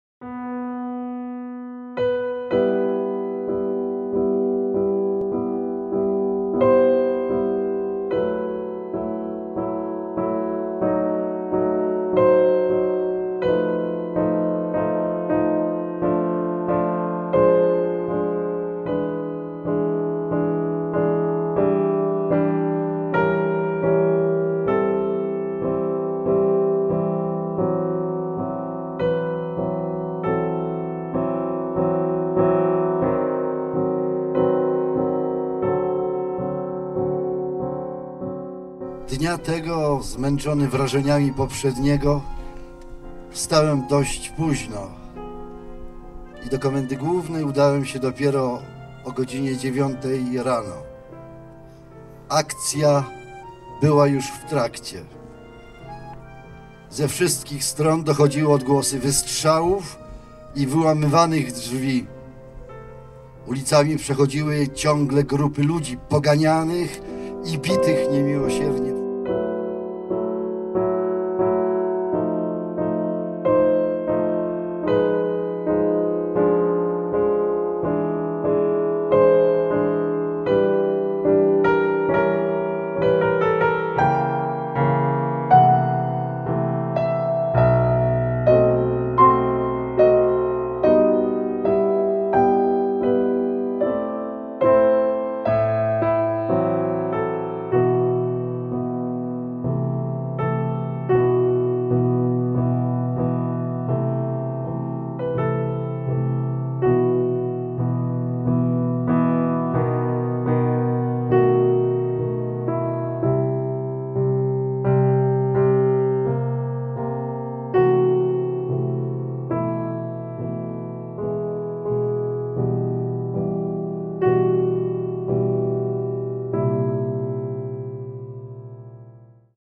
A diary from the Bialystok ghetto
A small notebook saved from waste paper... 70 pages describing the daily life in the ghetto in Bialystok... Dawid Szpiro begins writing down his memories at the age of 18.